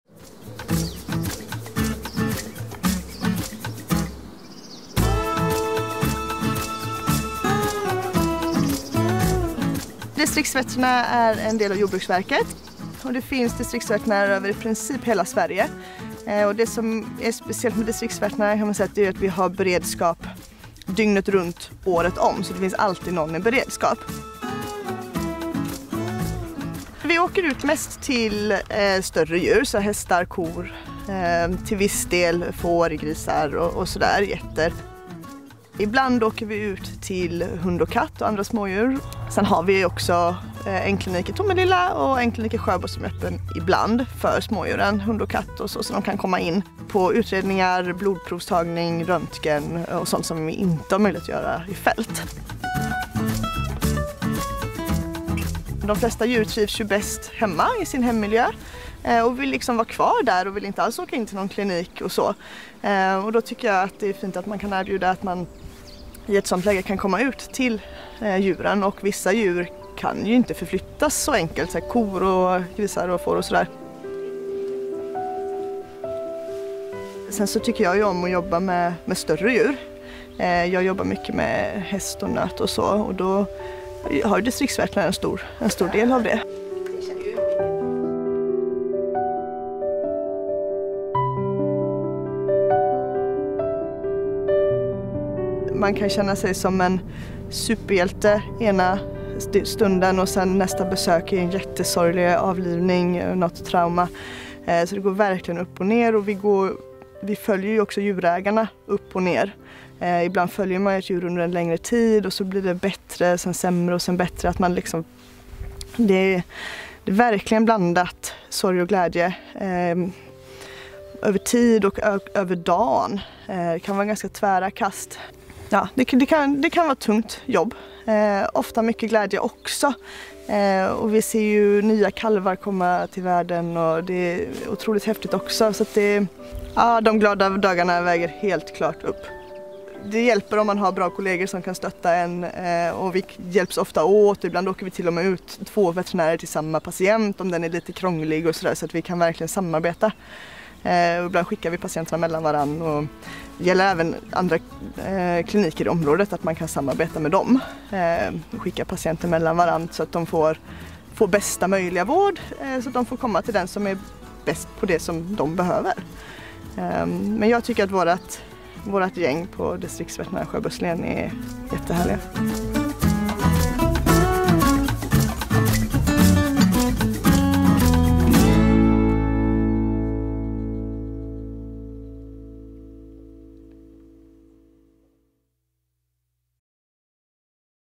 Österlensvepet del 18. På besök hos Distriktsveterinärerna
Här får vi följa med veterinären My som berättar om sitt yrke. Arbetsdagar fyllda av spänning och dramatik, men också av hårt slit, goda kollegor och stor omsorg. Här följs sorgen och glädjen åt och hör till vardagen. Men det är de glada dagarna som "helt klart väger upp", berättar My.